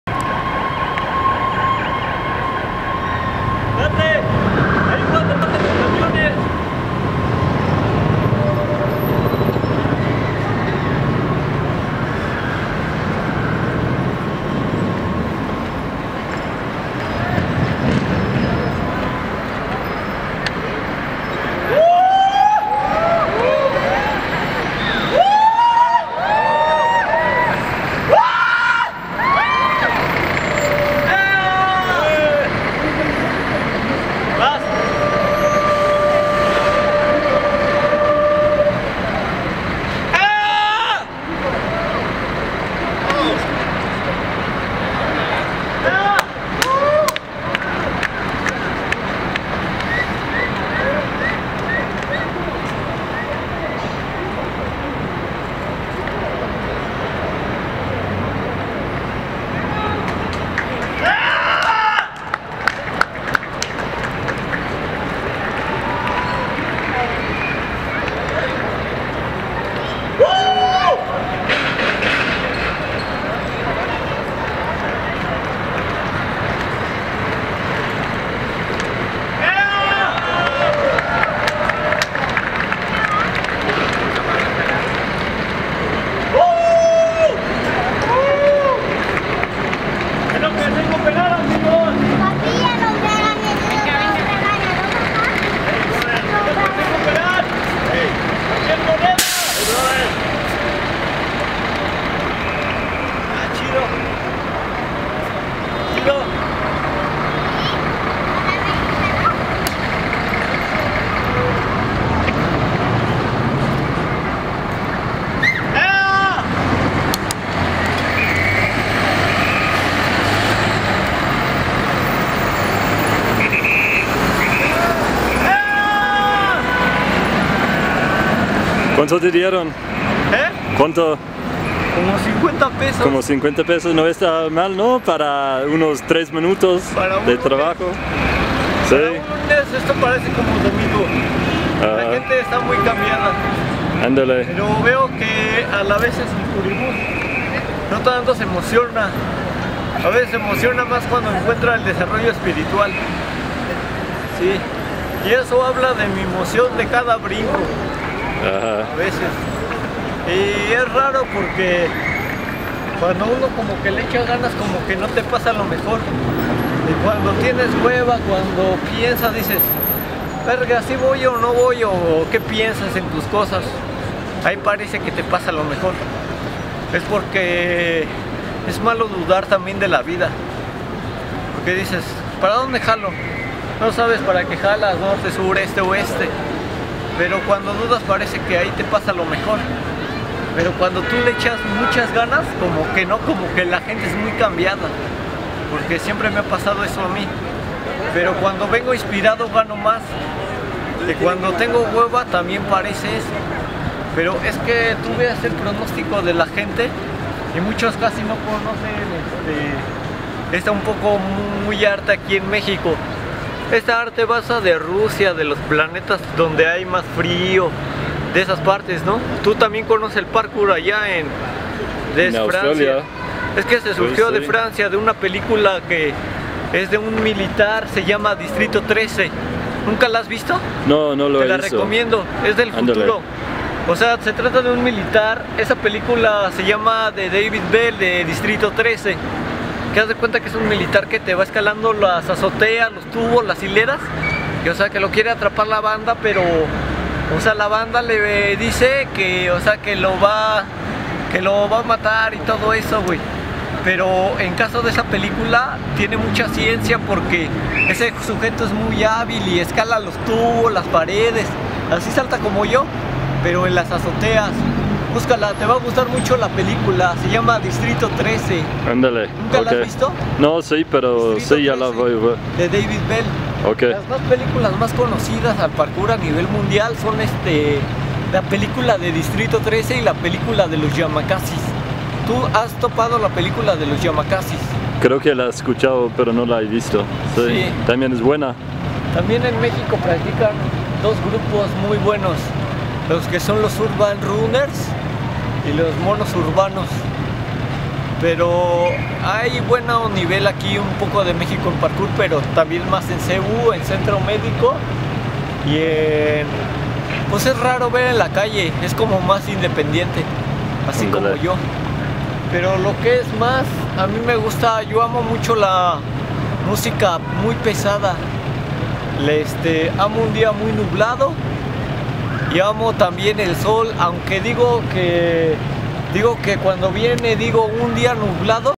Chapulin Brincando + short chat
Maestro jumper in the Mexico City downtown